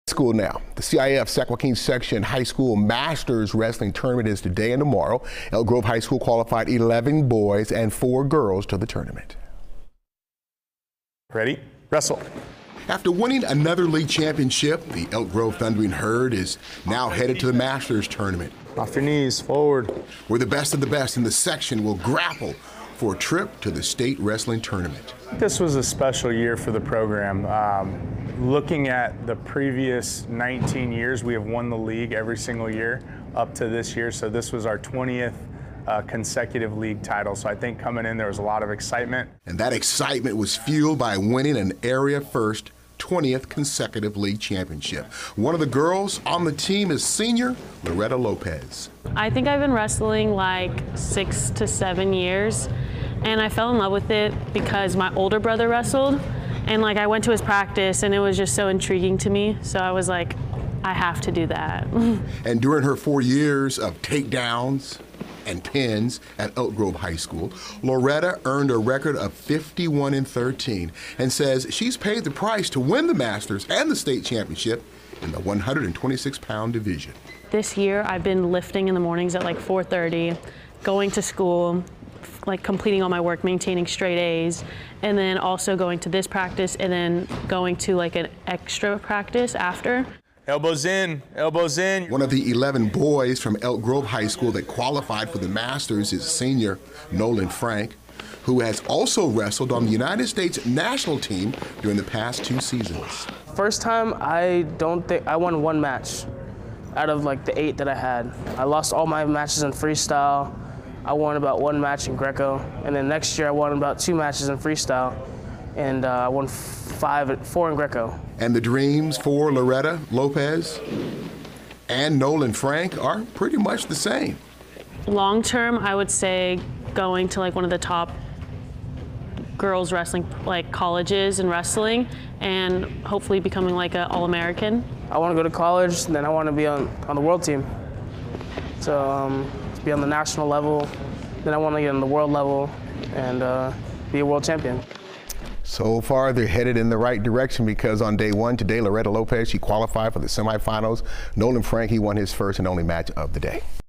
‘A lot of excitement’: 11 boys, 4 girls from Elk Grove HS wrestling team head to Masters tournament
After winning another league championship the Elk Grove Thundering Herd headed to the Masters tournament over the weekend.   Eleven boys and four girls from Elk Grove High School qualified for the Sac Joaquin Section High School Masters wrestling tournament, where the best of the best grappled for a trip to the state wrestling tournament.   “This was a special year for the program. Looking at the previous 19 years, we’ve won the league every single year up to this year. So this was our 20th consecutive league title,” said head coach Nick Jordan. “So I think coming in there was a lot of excitement.”  See more in the video above.